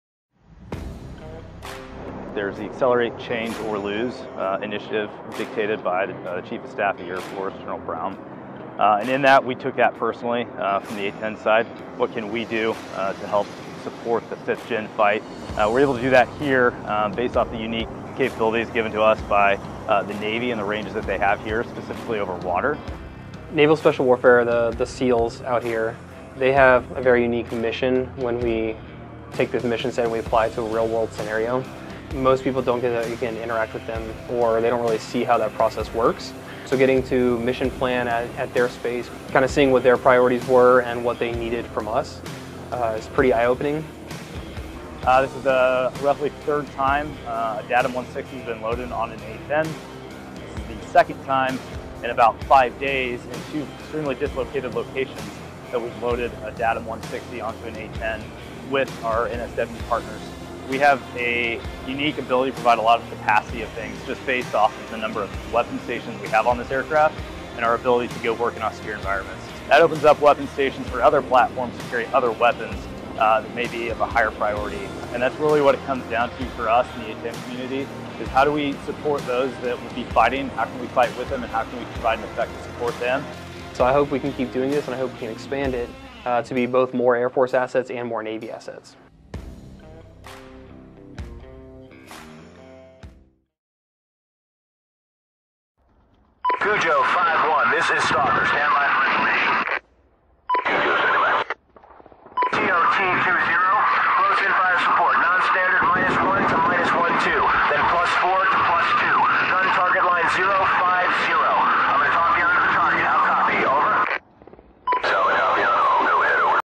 Green Flag West 23-02
Nov. 9, 2022 - Capt. Joseph Cole, 549th Combat Training Squadron Green Flag West Team Chief, and Maj. Taylor Raasch, 66th Weapons Squadron Instructor/Project Officer, speak about the Demo ATM-160 Miniature Air-Launched Decoy (MALD) use in conjunction with the A-10 during Green Flag-West 23-02 at Naval Air Station North Island, California. The DATM-160 was loaded onto an A-10 with participation from Naval Special Warfare operators.

Film Credits
Airman 1st Class Josey Blades